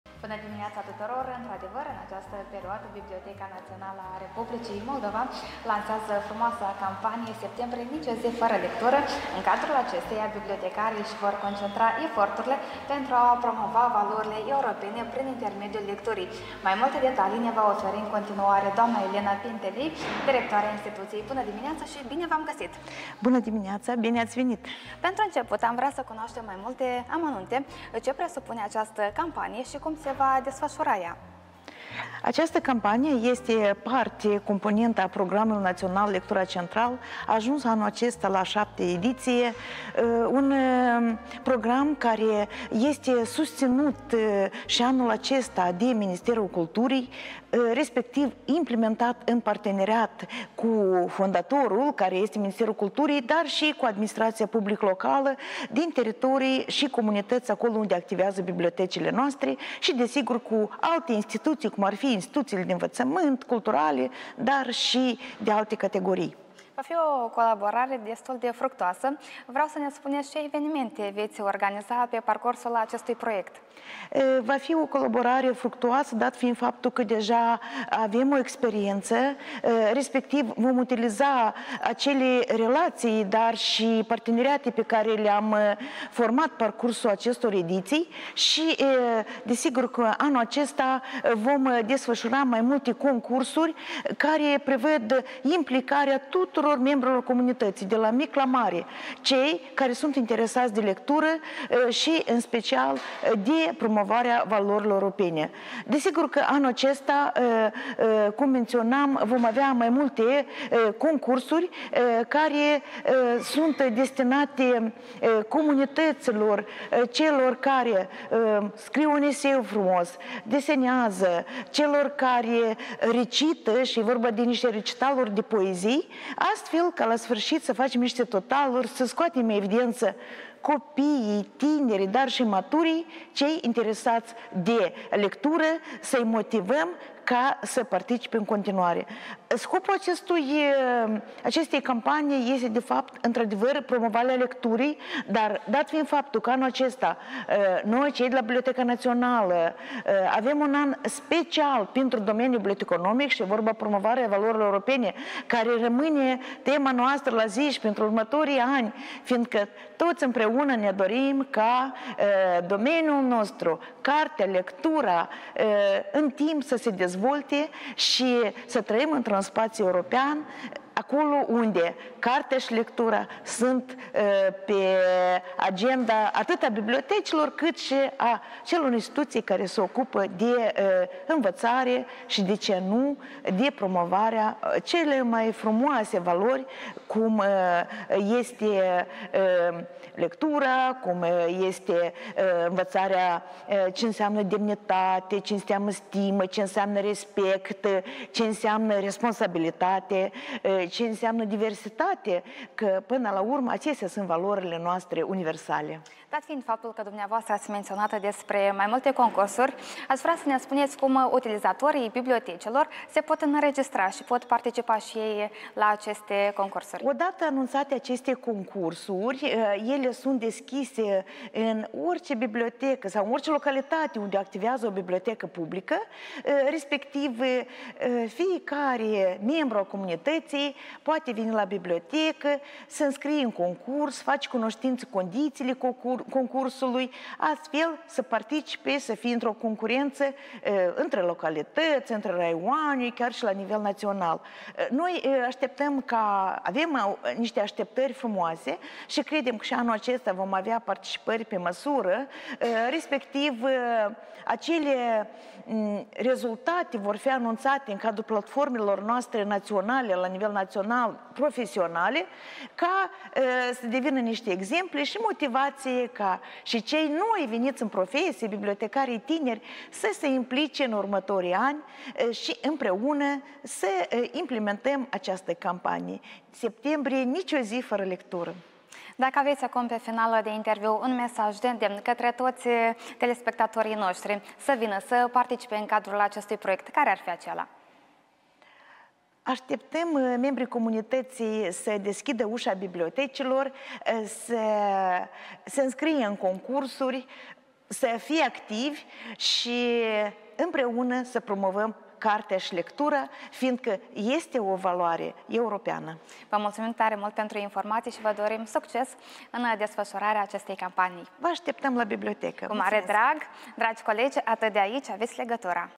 „Septembrie - Nici o zi fără lectură”
Biblioteca Națională lansează o campanie de promovare a lecturii. Mai multe detalii aflăm de la Elena Pintilei, Director General al BNRM.